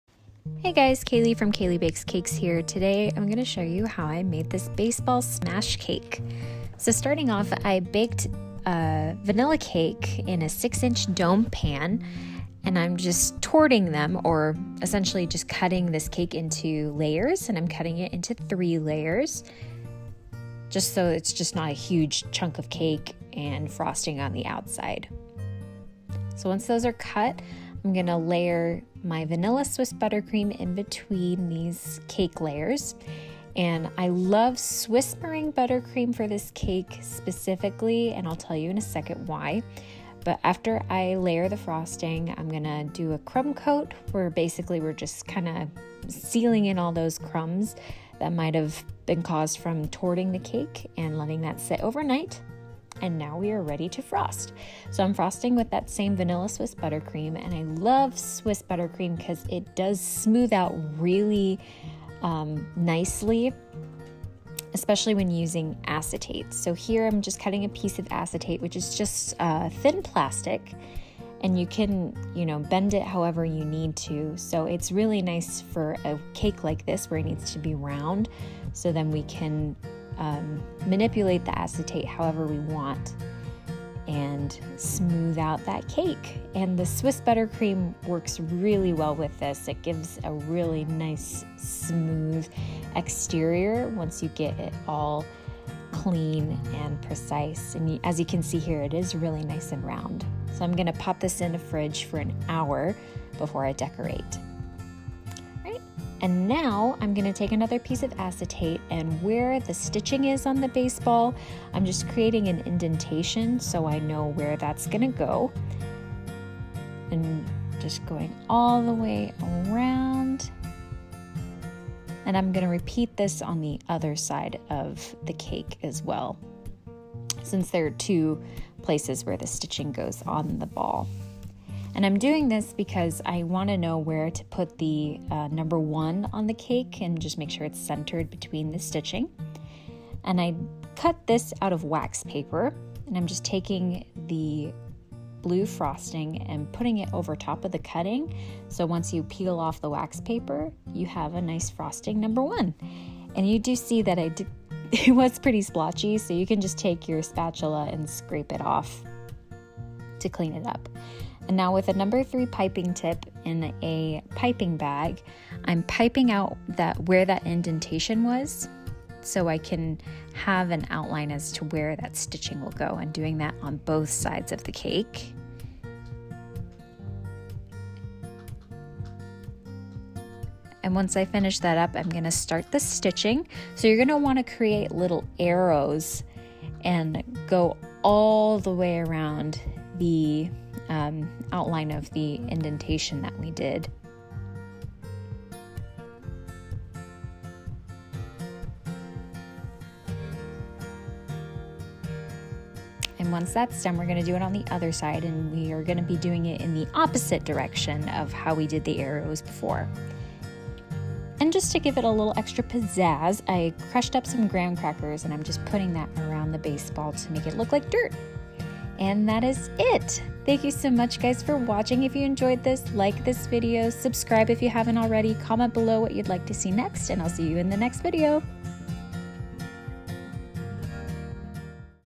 Baseball Smash Cake Tutorial | Kayli Bakes Cakes
In this tutorial, I show you how I made this 6" domed cake into a baseball smash cake.

The cake was my base vanilla cake with vanilla Swiss Buttercream and all of the detailing was made with dyed vanilla Swiss buttercream.

Dr. drer & arc posse